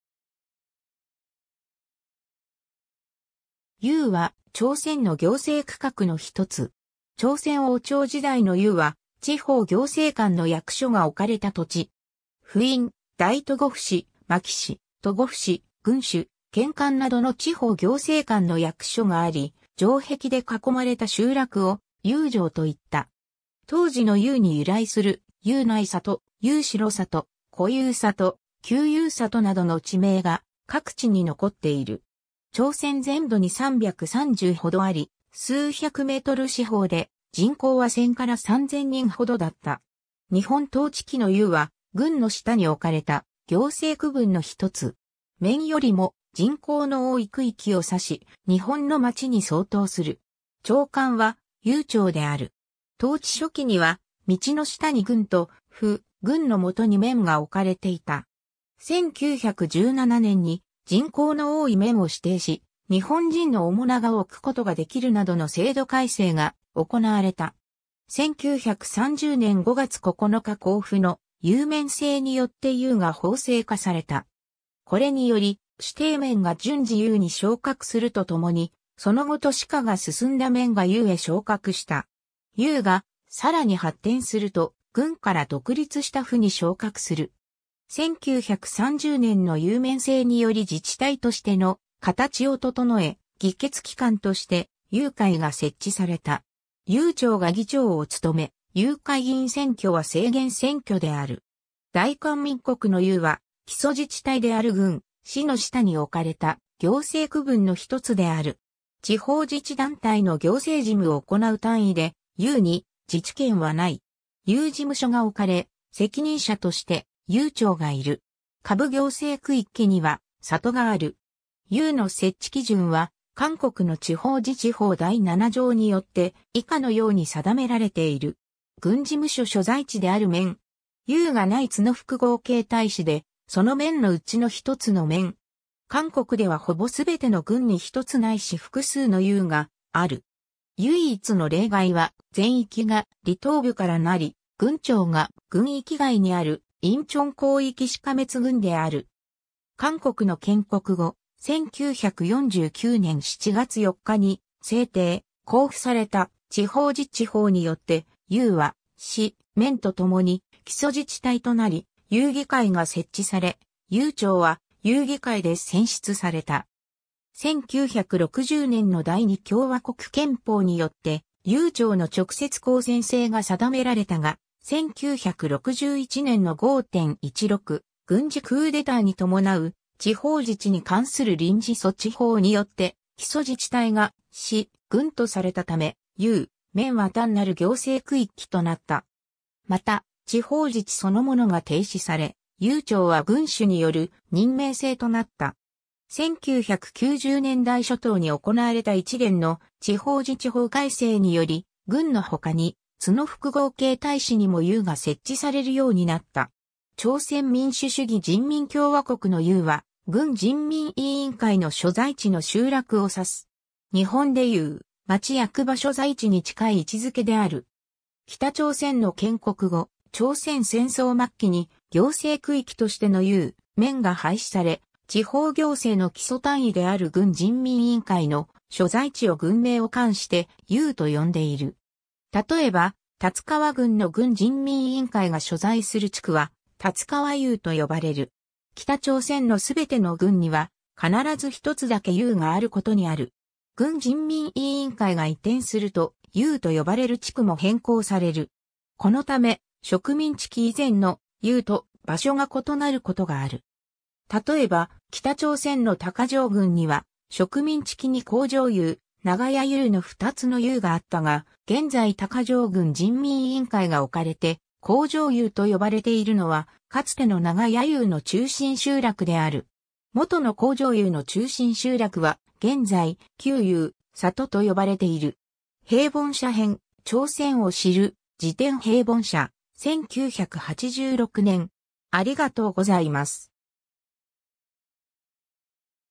邑 (朝鮮)
邑（ウプ）は、朝鮮の行政区画（行政区分）の一つ。
 朝鮮王朝時代の邑は、地方行政官の役所が置かれた土地。
府尹・大都護府使・牧使・都護府使・郡守・県監などの地方行政官の役所があり、城壁で囲まれた集落を邑城と言った（たとえば東莱邑城、海美邑城）。
当時の邑に由来する邑内里・邑城里・古邑里・旧邑里などの地名が各地に残っている。
朝鮮全土に330ほどあり、数百ｍ四方で、人口は1000～3000人ほどだった。
 日本統治期の邑は、郡の下に置かれた行政区分の一つ。
面よりも人口の多い区域を指し、日本の町に相当する。
長官は邑長である。
 統治初期には、道の下に郡と府（都市部、特に日本人の比率が高い地域）、郡の下に面が置かれていた。
1917年に人口の多い面を指定し、日本人の面長を置くことができるなどの制度改正が行われた（指定面）。
1930年5月9日公布の邑面制（昭和5年制令第12号）によって邑が法制化された。
これにより、指定面が順次邑に昇格するとともに、その後都市化が進んだ面が邑へ昇格した。
邑がさらに発展すると、郡から独立した府に昇格する。
 1930年の邑面制により自治体としての形を整え、議決機関として邑会が設置された。
邑長が議長を務め、邑会議員選挙は制限選挙である。
 大韓民国の邑は、基礎自治体である郡・市の下に置かれた行政区分の一つである。
地方自治団体（郡・市）の行政事務をおこなう単位で、邑に自治権はない。
邑事務所が置かれ、責任者として邑長がいる。
下部行政区域には里がある。
 邑の設置基準は、韓国の地方自治法第7条によって以下のように定められている。
 郡事務所所在地である面。
 邑がない都農複合形態市で、その面のうちの1つの面。
 韓国ではほぼすべての郡に1つないし複数の邑がある。
唯一の例外は、全域が離島部からなり郡庁が郡域外にある仁川広域市甕津郡である。
 韓国の建国後、1949年7月4日に制定・公布された地方自治法（8月15日施行）によって、邑は市・面とともに基礎自治体となり、邑議会が設置され、邑長は邑議会で選出された。
1960年の第二共和国憲法によって邑長の直接公選制が定められたが、1961年の5・16軍事クーデターにともなう「地方自治に関する臨時措置法」によって基礎自治体が市・郡とされたため、邑・面は単なる行政区域となった。
また、地方自治そのものが停止され、邑長は郡守による任命制となった。
 1990年代初頭に行われた一連の地方自治法改正により、郡のほかに都農複合形態市にも邑が設置されるようになった。
 朝鮮民主主義人民共和国の邑は、郡人民委員会の所在地の集落を指す。
日本でいう町役場所在地に近い位置づけである。
北朝鮮の建国後、朝鮮戦争末期に行政区域としての邑・面が廃止され、地方行政の基礎単位である郡人民委員会の所在地を郡名を冠して邑と呼んでいる。
たとえば龍川郡の郡人民委員会が所在する地区は「龍川邑」と呼ばれる（通常は「龍川郡 邑」「룡천군 읍」と表記される）。
北朝鮮のすべての郡には必ず1つだけ邑があることにある。
 郡人民委員会が移転すると、邑と呼ばれる地区も変更される。
このため、植民地期以前の「邑」と場所が異なることがある。
たとえば、北朝鮮の高城郡には植民地期に高城邑・長箭邑の2つの邑があったが、現在高城郡人民委員会がおかれて「高城邑」と呼ばれているのはかつての長箭邑の中心集落である。
元の高城邑の中心集落は、現在「旧邑里」と呼ばれている。
 ^ 平凡社編『朝鮮を知る事典』平凡社、1986年 ^ （朝鮮語） 지방자치법